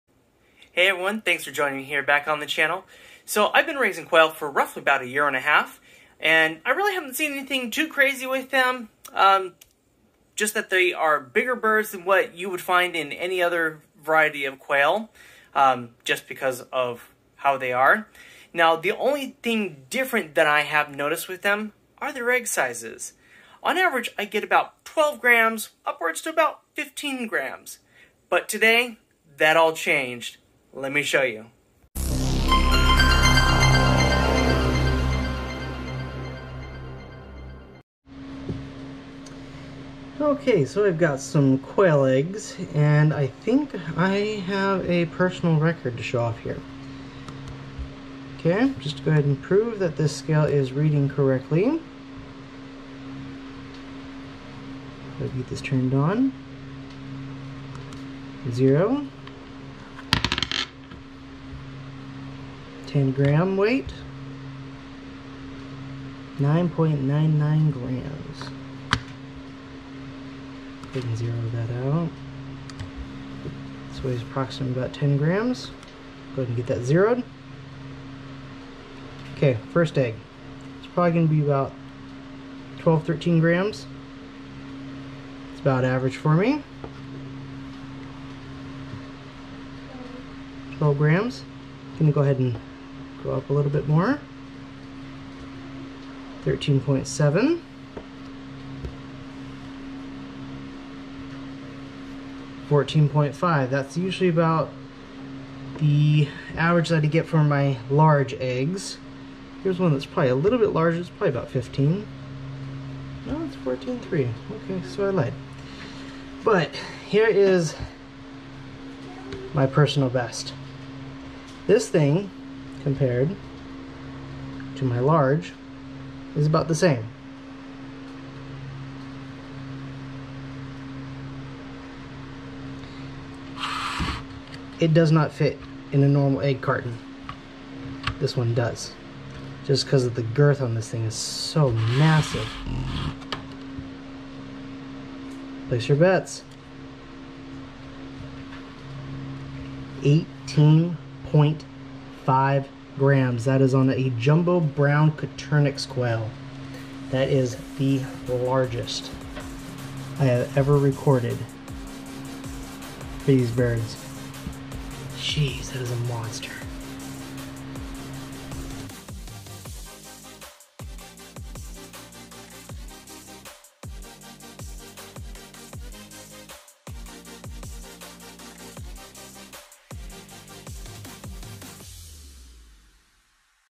Jumbo Brown Coturnix Quail Egg - This Thing Is Huge!
I've seen my share of quail eggs from: normal to large and fairy eggs to gelatin orbs, but this one takes the cake!

When packaging my 15g eggs, it's a little snug closing the container and I have to leave a couple of snap bubbles  (I don't know the technical term 😆) unbuttoned. This 18g one was so big it wouldn't fit in the container or let me close it!

What are your personal records?

Scale used
Egg cartons used